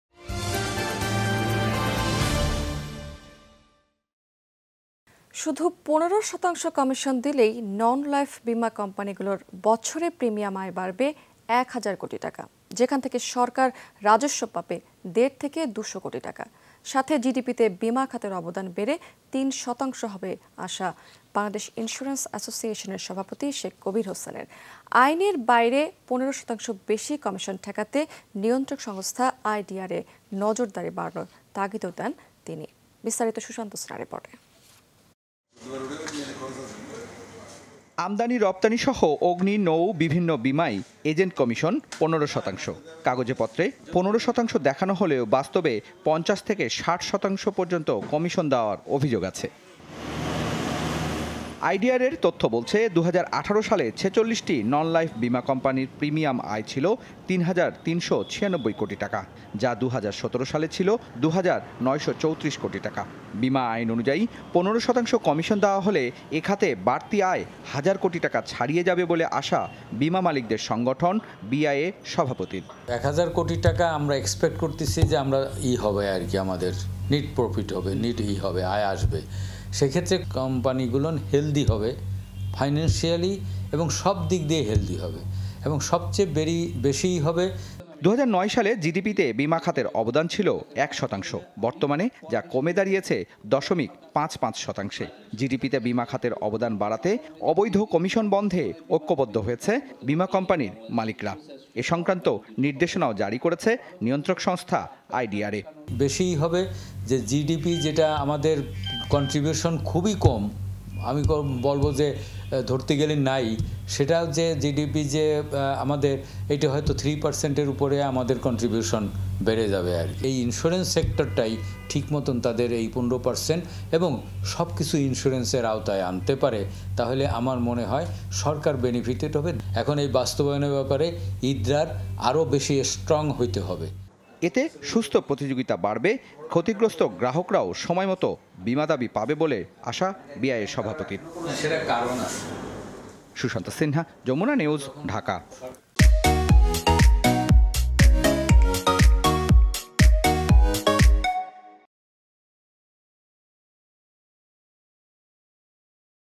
১৫% কমিশনে অনিয়ম কমবে বীমা খাতে | Jamuna TV
শুধু ১৫ শতাংশ কমিশন দিলে, নন লাইফ বীমা কোম্পানীগুলোর বছরে প্রিমিয়াম আয় বাড়বে এক হাজার কোটি টাকা। যেখান থেকে সরকার রাজস্ব পাবে দেড় থেকে দু'শ কোটি টাকা। সাথে জিডিপিতে বীমাখাতের অবদান বেড়ে ৩ শতাংশ হবে আশা বাংলাদেশ ইন্সুরেন্স অ্যাসোসিয়েশনের সভাপতি শেখ কবির হোসেন। আইনের বাইরে ১৫ শতাংশ বেশি কমিশন ঠেকাতে নিয়ন্ত্রক সংস্থা আইডিআরএ নজরদাড়ি বাড়ানোর তাগিদও দেন তিনি।বিস্তারিত সুশান্ত সিনহার প্রতিবেদনে।